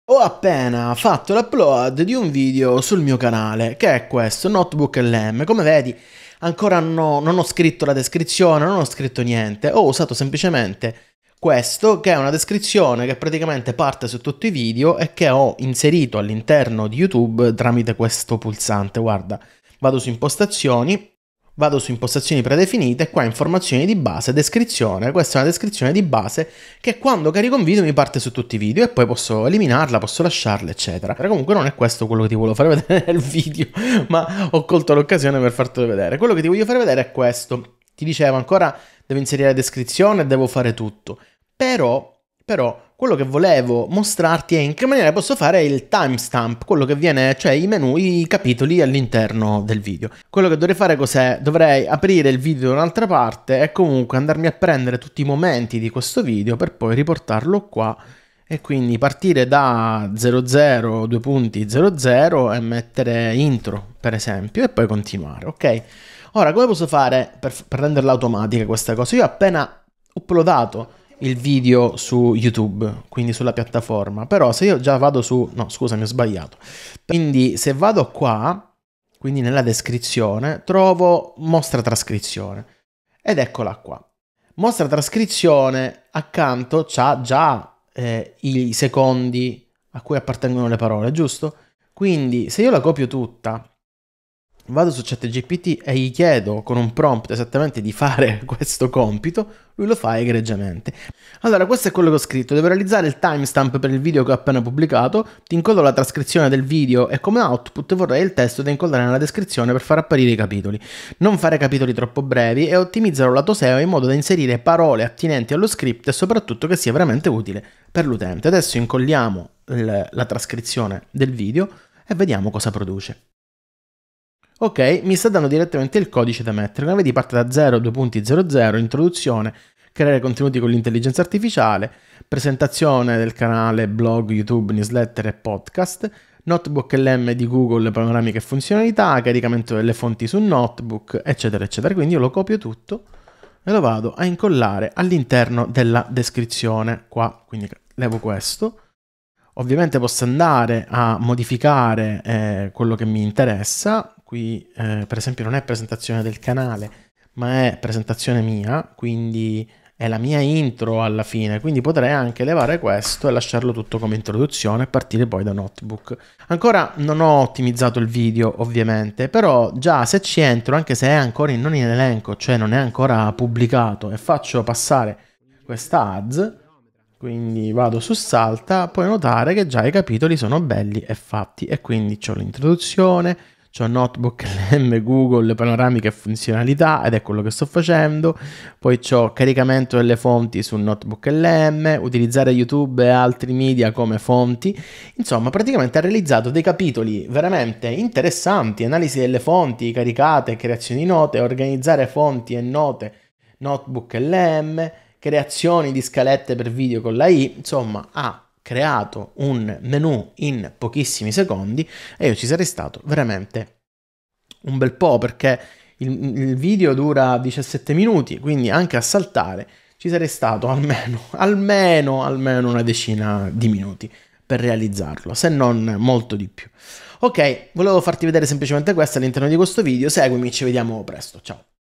Capitoli per video YouTube in automatico usando l'IA - Pazzesco!
In questo video ti mostro come creare automaticamente i timestamp (capitoli) per i tuoi video YouTube utilizzando ChatGPT e Notebook LM. Vedrai come impostare una descrizione predefinita per ogni video che carichi, in modo da risparmiare tempo nella fase di upload, e come sfruttare la funzionalità di trascrizione automatica di YouTube per generare facilmente i capitoli del video.
Utilizzando ChatGPT, potrai ottenere in pochi secondi un elenco di timestamp ben ottimizzati per migliorare la visibilità dei tuoi contenuti, rendendoli più utili e navigabili per gli utenti. Se hai mai passato troppo tempo cercando di creare capitoli manualmente, questo tutorial ti mostrerà come velocizzare il processo e sfruttare al meglio le potenzialità dell'intelligenza artificiale.
Se ti interessa migliorare la tua produttività su YouTube e creare contenuti di qualità con meno sforzo, questo video fa al caso tuo. Seguimi e ci vediamo nel prossimo video!

0:00 Introduzione
0:44 Come impostare una descrizione predefinita per i video
1:08 Mostrare la trascrizione del video su YouTube
1:31 Creazione automatica dei timestamp con ChatGPT
2:05 Esempio di timestamp per un video
2:48 Ottimizzazione e inserimento dei capitoli su YouTube
3:26 Guardiamo in risultato
3:47 Conclusione e invito a iscriversi
👑 SITO E SERVIZI

🧿 LA MIA ATTREZZATURA: